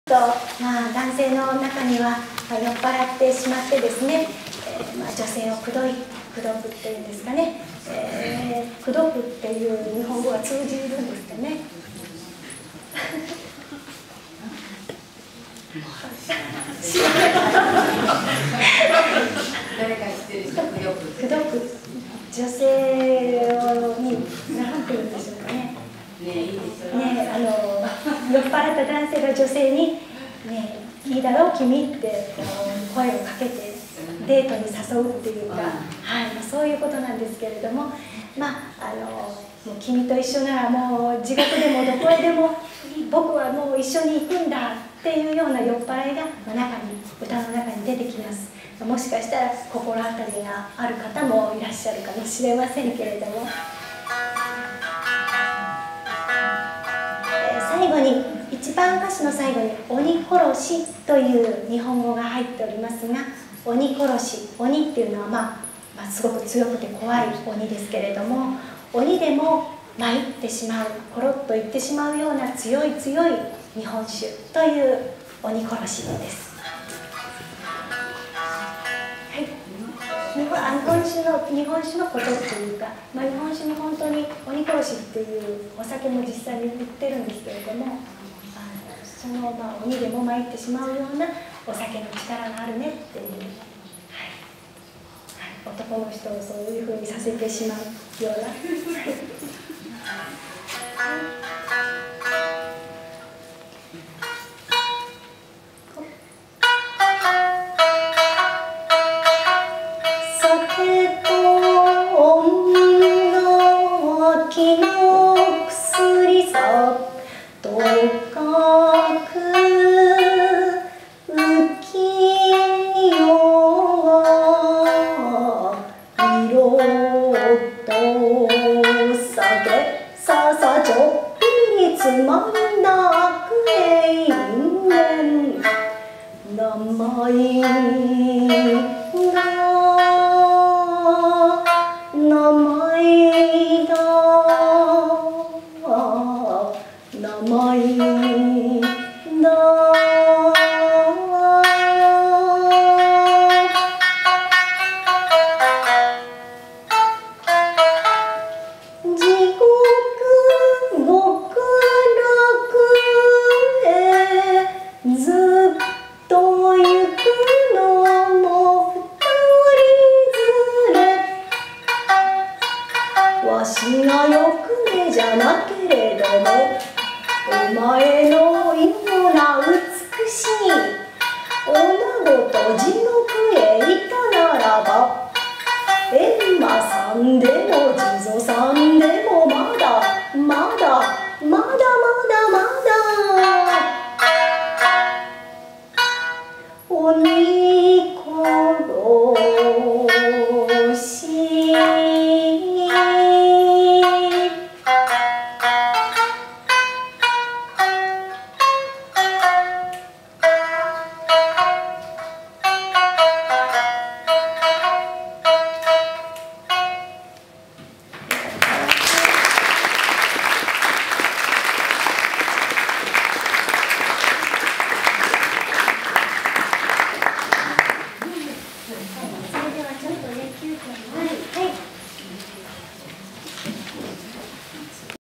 Hiyama Umekichi - The Art Of Geisha @ University Vienna
Since the 19th Century the mountain "Fujiyama", the Cherry blossoms and the Geisha became the three most famous symbols of Japan in western societys.
The term "Geisha" can be translated as "Artistic Person". The role of the Geisha is not just to look beatiful in her precious Kimono but to control several instruments, sing various songs, be good at dancing and be an excellent, professinal hostess in a traditional and elegant way who serves her guests and entertains them with her artistic skills on drinking festivals.